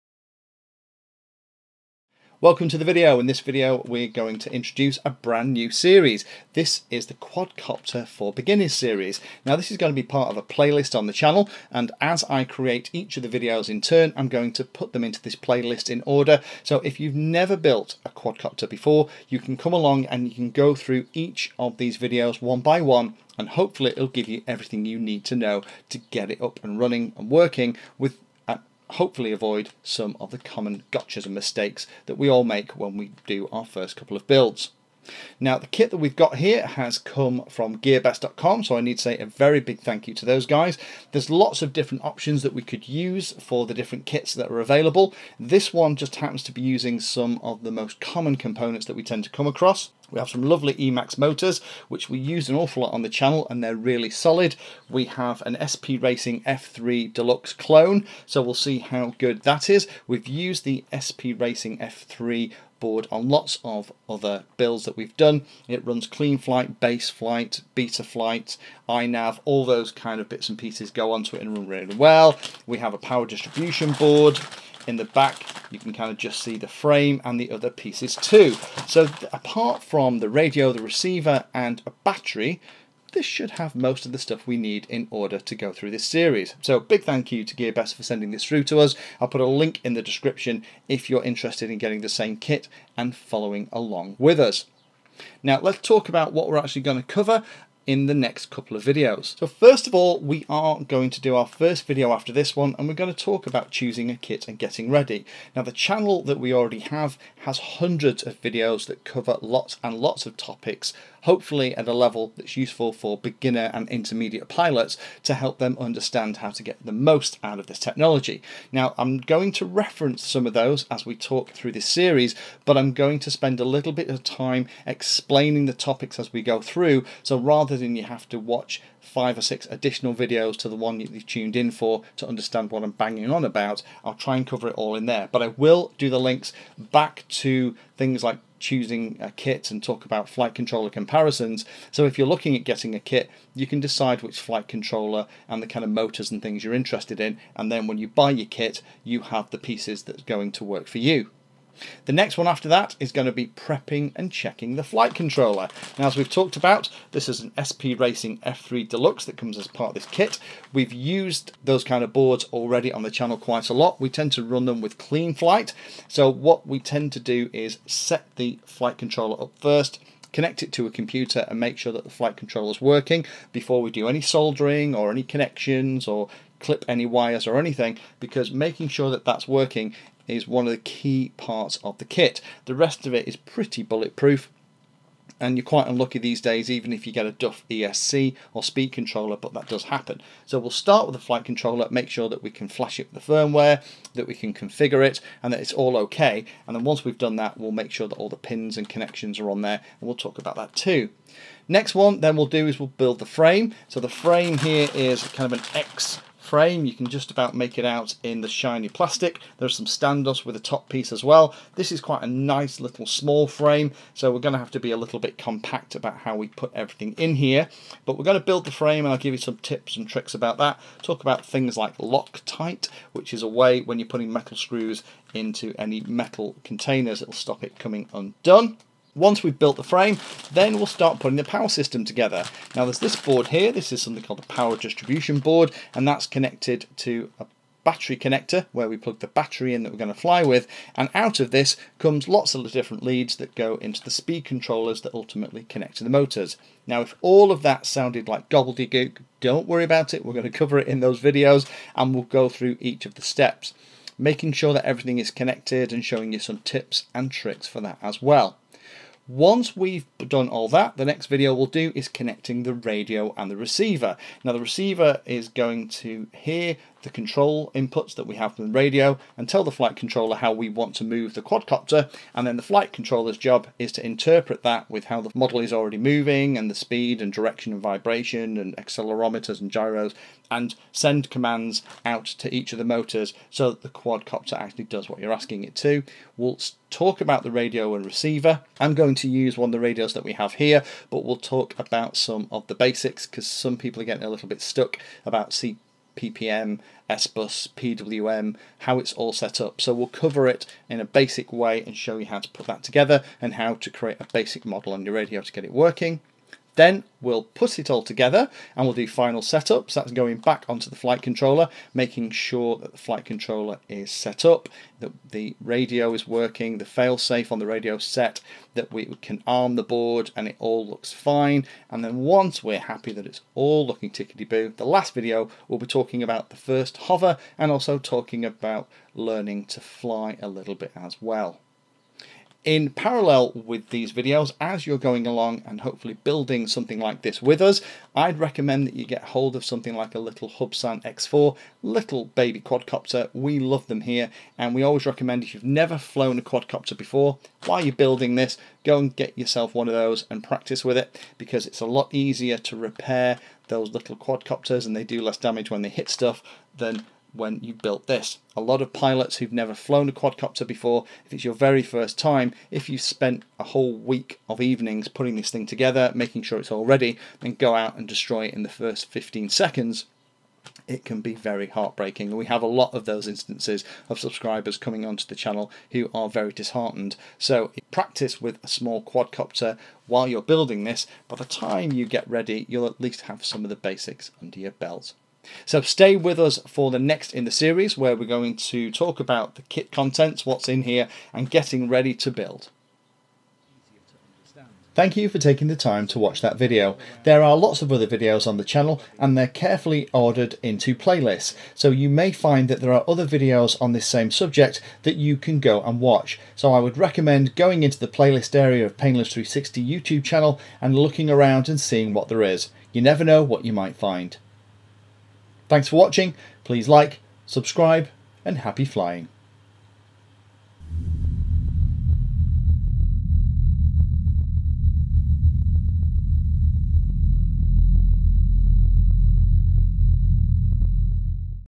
(1/9) Quadcopter building for beginners: Introduction (kit from Gearbest.com)
In this new series we will build this kit, setting up the flight controller, building the frame, adding the power system, wiring up the ESCs and motors, connecting the radio and then doing final checks and then having the first test hover!

This is a series designed for the very new builder and will take more time than in our other videos to show and explain every step and three letter acronym.

Happy flying!